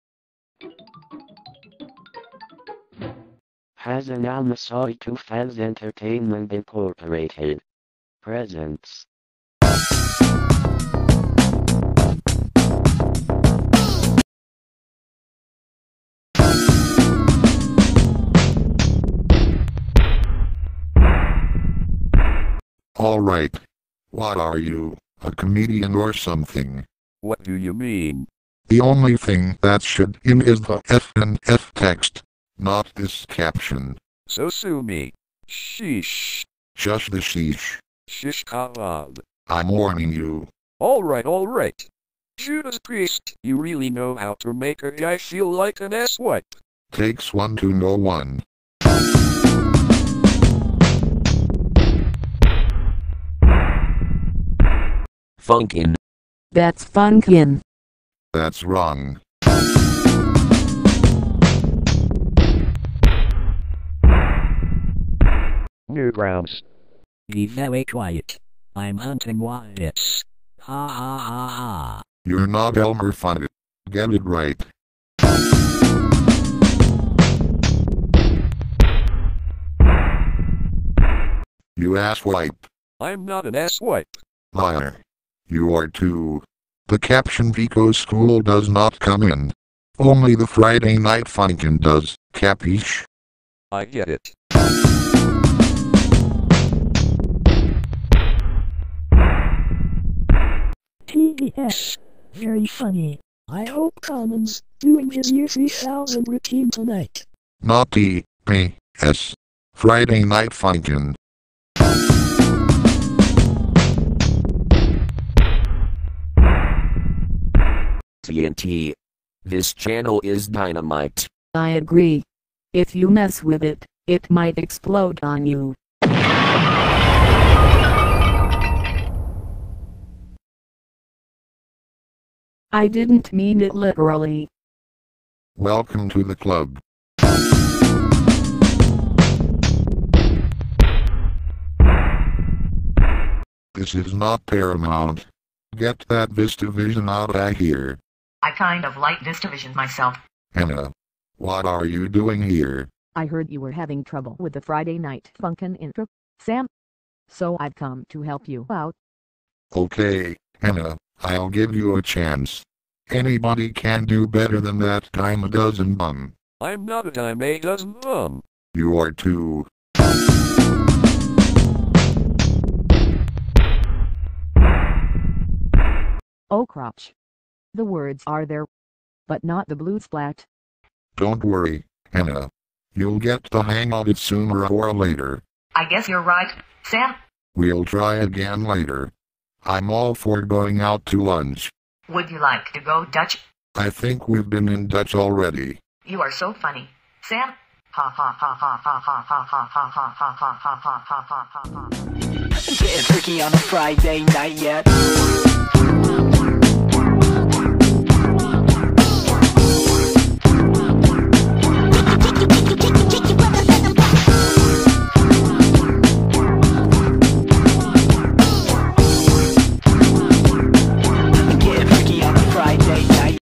Friday Night Funkin' Intro Bloopers Episode 14: Everything But the Text
the fourteenth episode of: FNFIB Series credit to:
davemadson films inc. and
Timothy Ly Productions Inc.
Starring:
Sam, Mike, Mary, Anna,
Text Guy, Jimmy, Hank,
Beulah and Abby
Tantrums: 1
made using:
Online Microsoft Sam TTS Generator,
voice changer with effects and Kinemaster